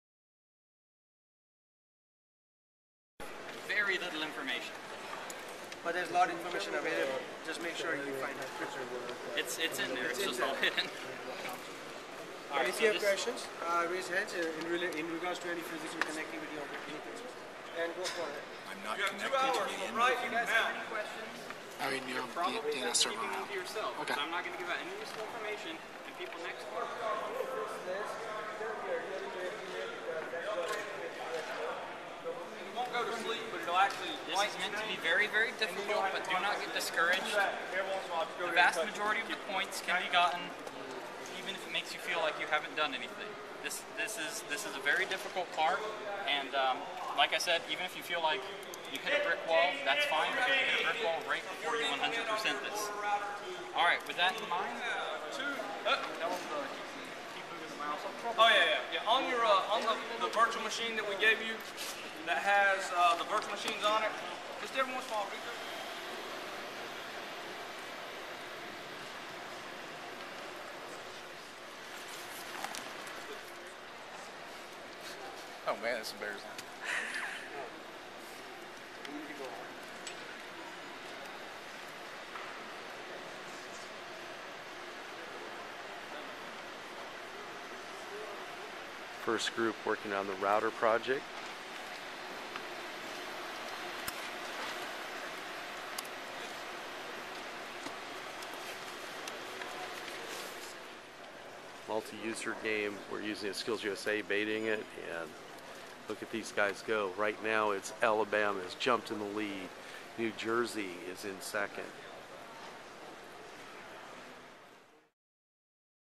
SkillsUSA Internetworking 2012 Day 2
Day 2 the routing and Switching Project, Netwrking, Wireless, and cable troubleshooting, Technical Assistance call, and the exam competitions begin Competition